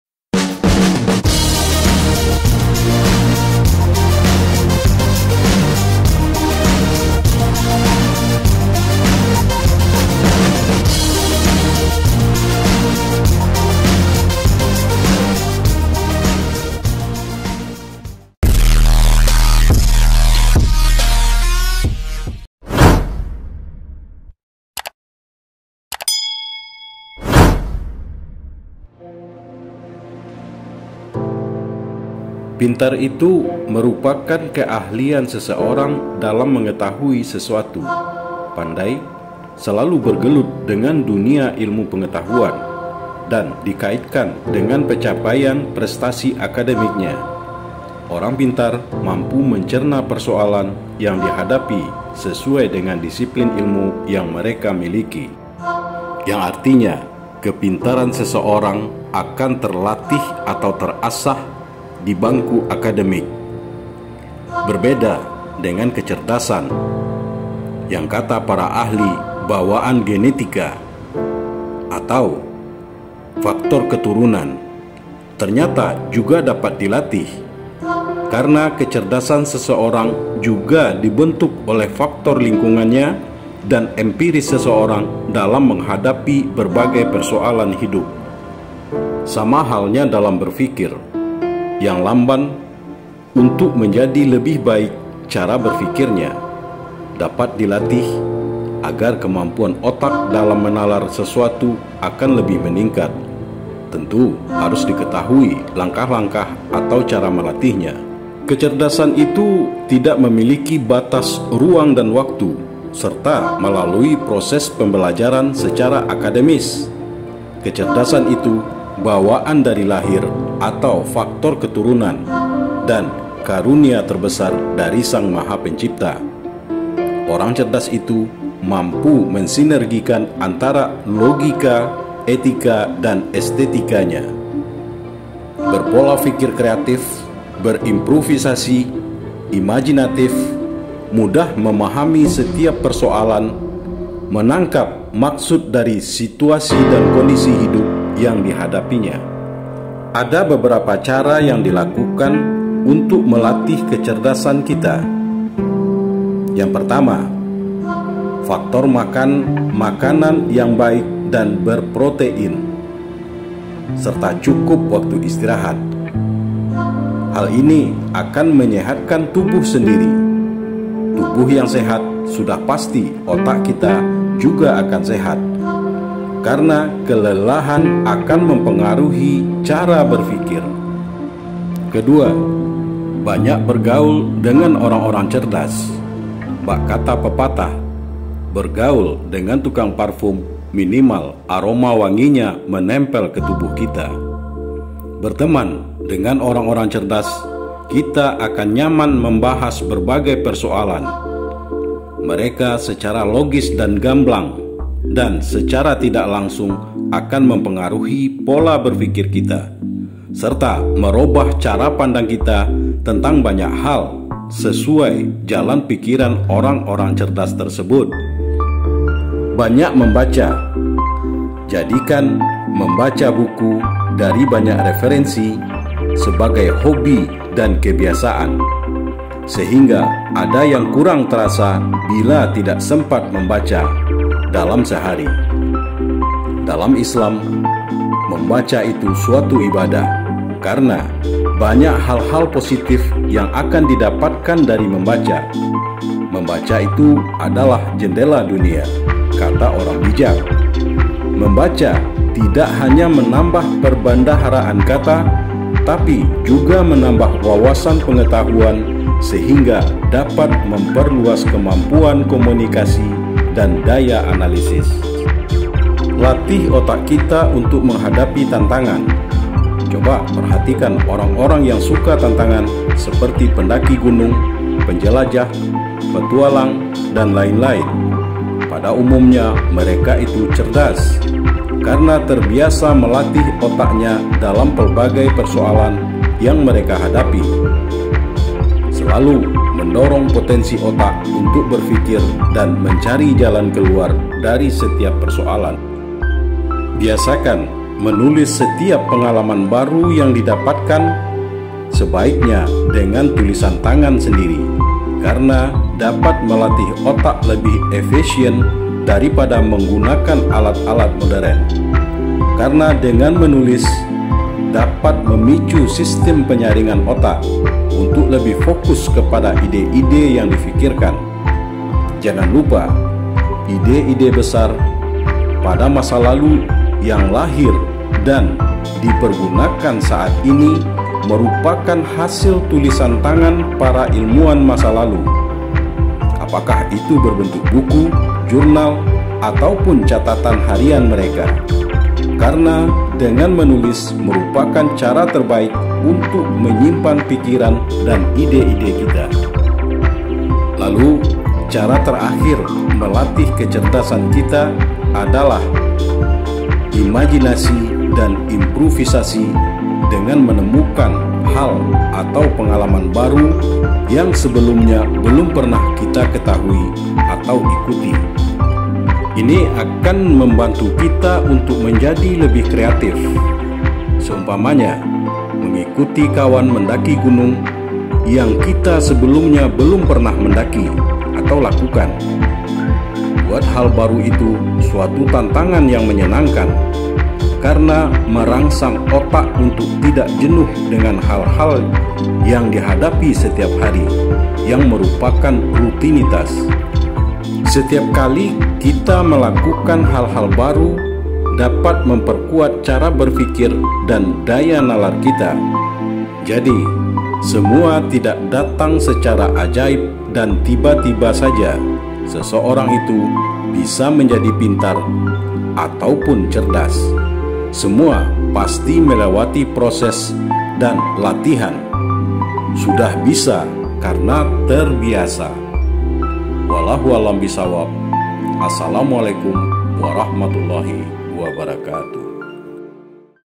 Cara Melatih Kecerdasan Otak
Orang yang dikatakan pintar itu pandai pasti pandai, dan memiliki pengetahuan serta menguasai ilmu yang dipelajari dari proses belajar baik formal maupun informal secara disiplin dan teratur. Sehingga dalam menghadapi suatu masalah serta mencari solusinya mengandalkan teori-teori yang dikuasainya dan orang pintar dapat diketahui dari prestasi akademiknya.

Sementara orang yang Cerdas itu cerdik dan cermat. Bisa memilih jalan pendek atau tidak berbelit dalam menghadapi suatu masalah. Teori-teori dari ilmu pengetahuan hanya sebagai pendukung karena dia lebih memilih secara logika dan substansial dalam mencari solusi.

Namun di samping kepintaran, kecerdasan yang merupakan bawaan dari lahir atau faktor genetika. Kecerdasan juga dapat dilatih untuk menjadi cerdas dengan mengikuti 6 langkah berikut:

1. Cukup istirahat dan memakan makanan yang berprotein.
2. Bergaul dengan orang-orang cerdas.
3. Banyak membaca.
4. Malatih otak dengan banyak permainan yang menantang.
5. Membiasakan menulis dengan tangan setiap pengalaman baru atau ide-ide yang didapat.
6. Mencari atau Menemukan pengalaman baru yang dapat meningkatkan daya imajinasi, kreativitas, dan improvisasi.

Semoga video ini dapat memberi manfaat
Kecerdasan Berfikir

Zulhasmi Channel